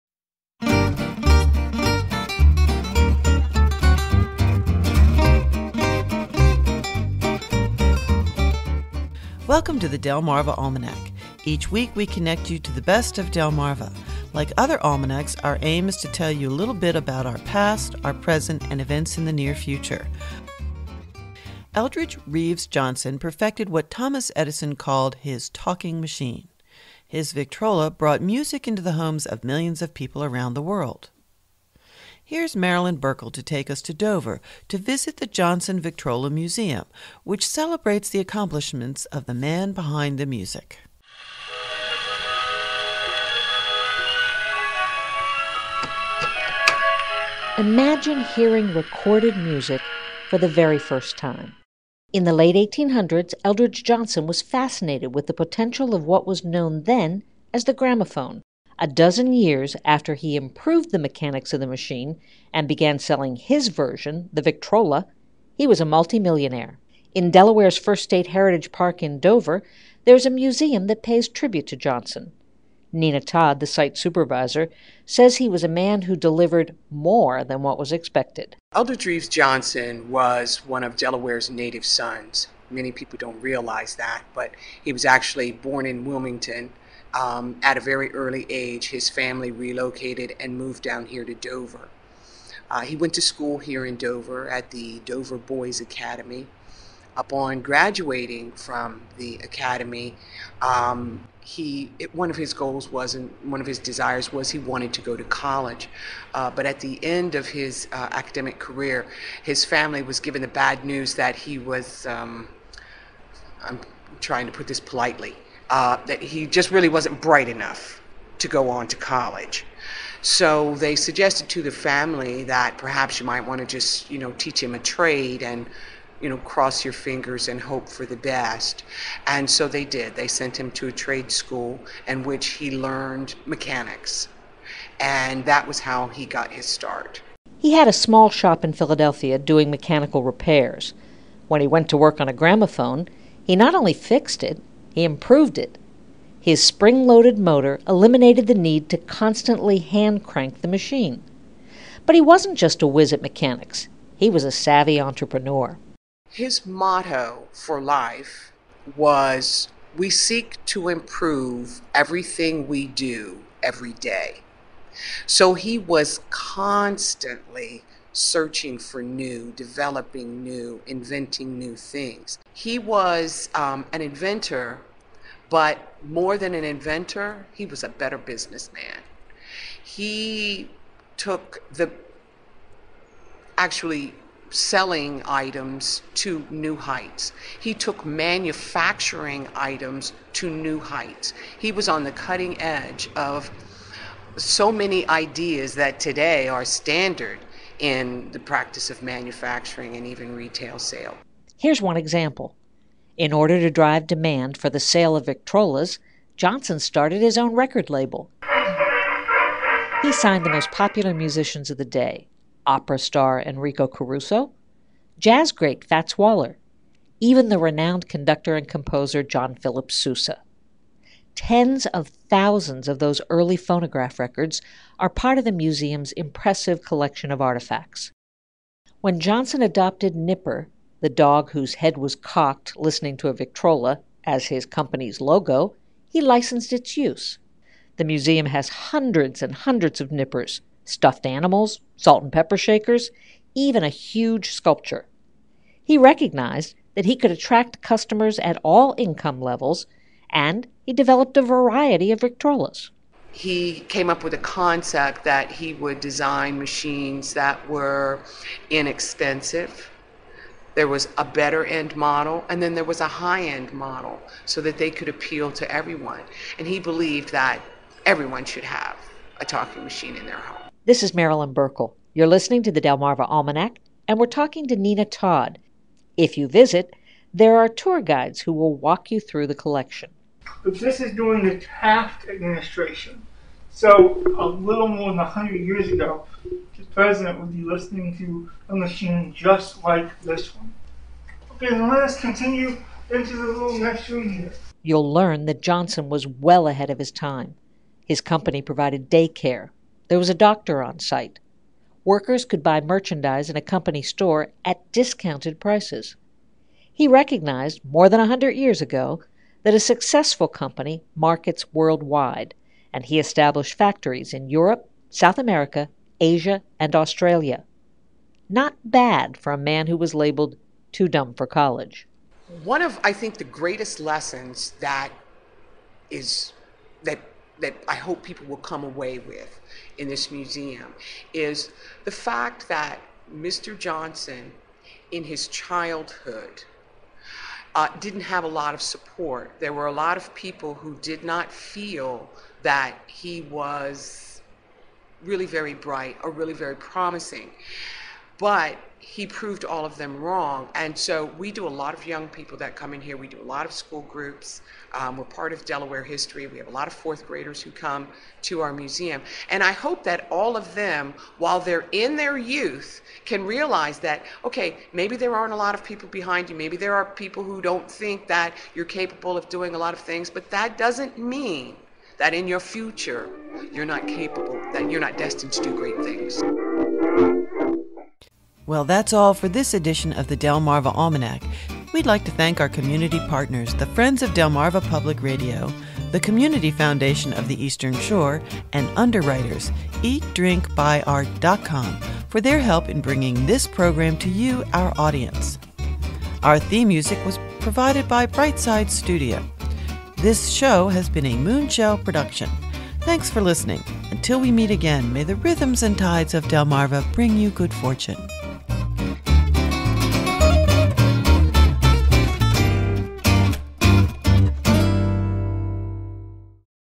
The Johnson Victrola Museum
Occasional Almanac contributor Marilyn Buerkle takes us to Dover to visit the Johnson Victrola Museum.